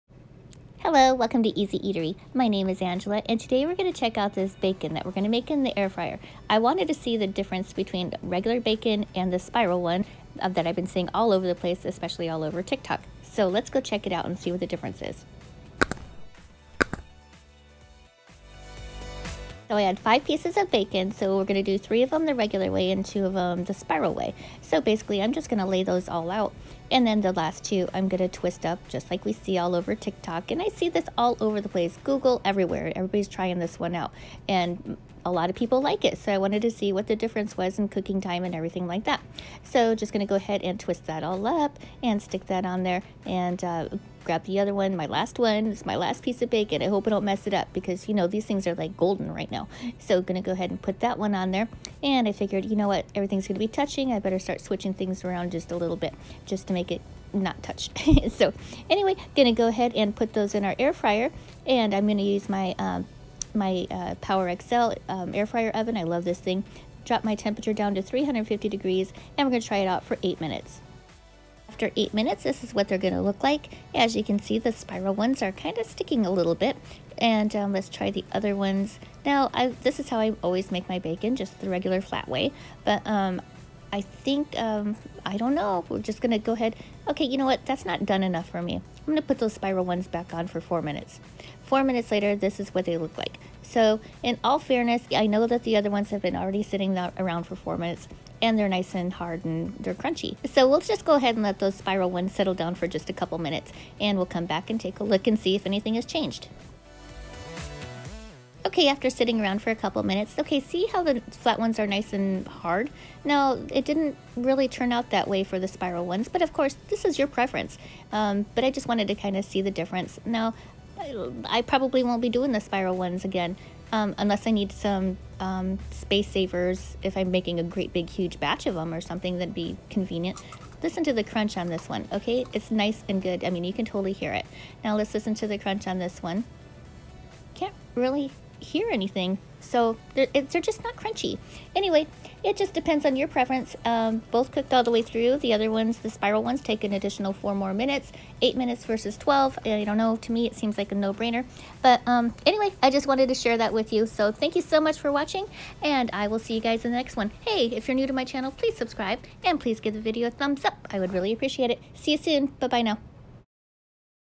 Spiral VS Regular Air Fryer Bacon | Which Is Better And Easier?
Hello, welcome to EZ Eatery.  Today I wanted to see what the difference between air fryer bacon, laying flat (the regular way) and what the excitement for the spiral, and viral, Tik Tok way was.  Now, I'm only using 5 pieces of bacon, because I certainly don't want to let anything go to waste in case I mess up.  Have you seen the price of bacon these days?  Sheesh!

I'm using 5 pieces of bacon.  3 to cook in the air fryer and 2 to try the spiral way.  I'm twirling up the 2 pieces and laying them side by side the other 3 pieces of bacon.  We're going to cook them in the Power XL 10 QT 1700 Watt Air Fryer Oven at 350 F for 8 minutes.

When I pulled them all out after 8 minutes, the spiral ones were quite limp and didn't look quite done enough for me.  So I put them back in for an additional 4 minutes.  Now, I like my bacon nice and crunchy, so after trying them out, I think I'm going to stick with the old fashioned lay-them-flat way.  It cooks in way less time in the air fryer and I just prefer the crunch.  Now, if I were going to make bacon for a bunch of people and was short on time, I would probably air fry them the spiral way.  Just to conserve space and have to cook in less batches.

There!!...that's my two cents and am happy to do the experiment for you.